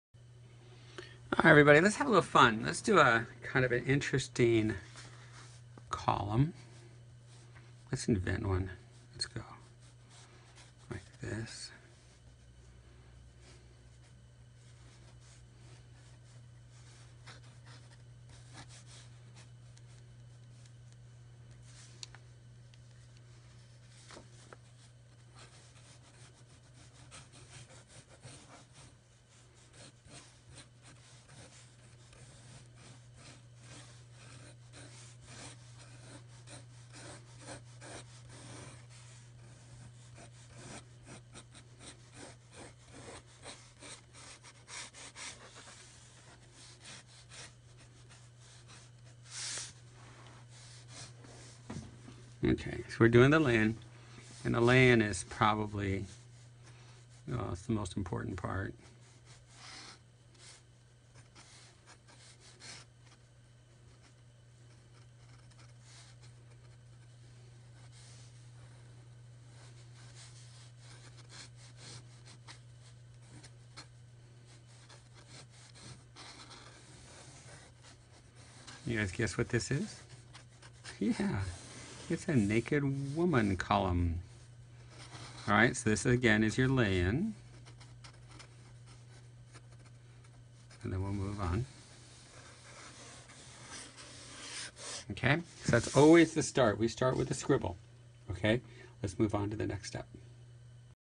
Drawing a Column part 5 - Column and Figure
In this exercise Sheldon starts with a new column and adds a figure.  The drawing is done only with a fountain pen and water brush to show how to do simple drawing with your fountain pen.
It's part of our Saturdays With series on our Facebook

The paper is Arches Text Wove and the journals are from our Innovative Journaling